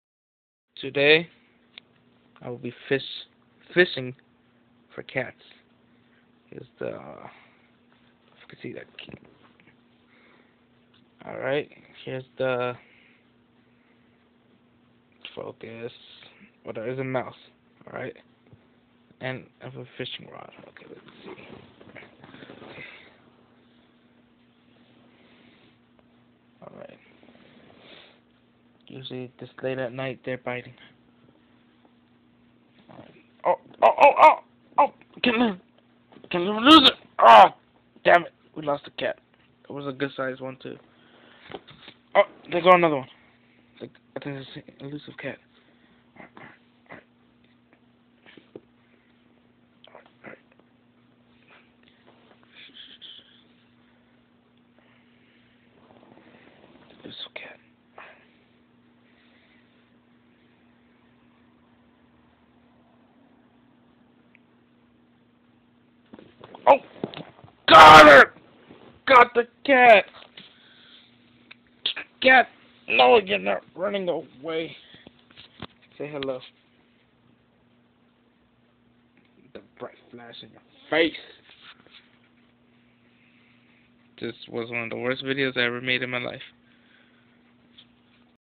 Fishing for Cats
We go fishing for catfish in one of my worst videos ever in my opinion.